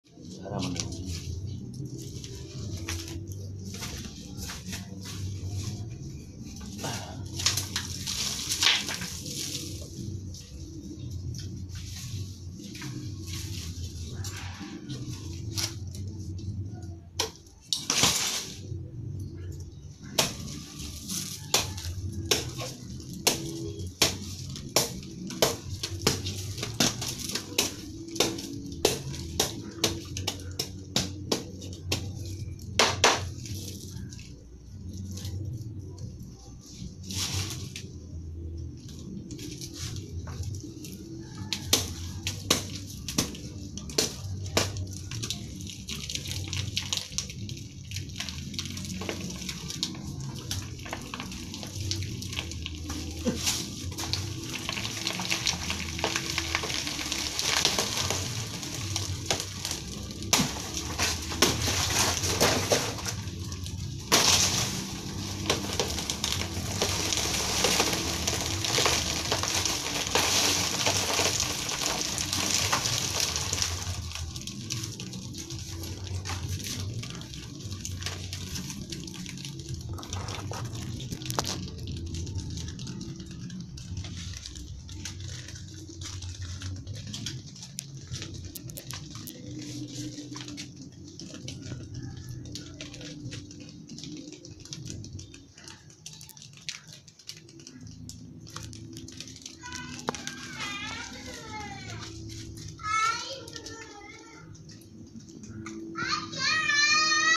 tutorial menebang buah pisang CAVENDISH pohon tinggi..
untuk memulai tahapan menanam pisang cavendish tidaklah susah cukup siapkan bibit yg sehat bebas dari penyakit bawaan induk. sebelum menanam dilahan persawahan upayakan bibit seterilisasi terlebih dahulu dengan menggunakan pupuk anti virus. lalu berikan pupuk kandang permentasi...